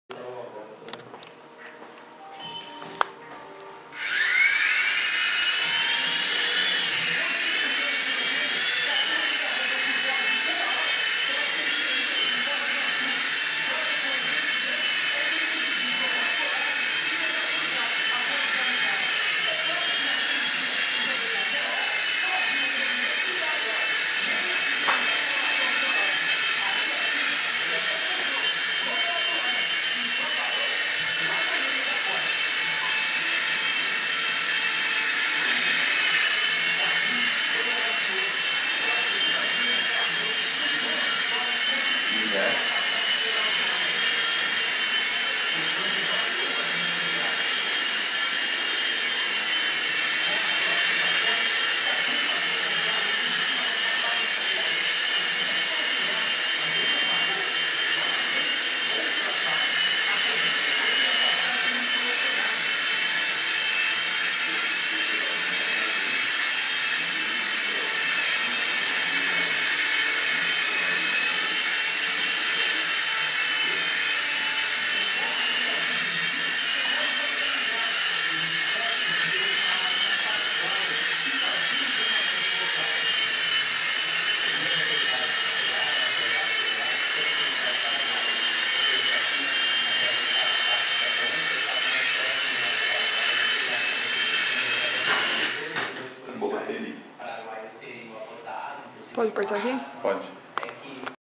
MAQ01302.MP4
Swift Mini Helicopter - Oscar Fernando Baradel Conceição - teste de voo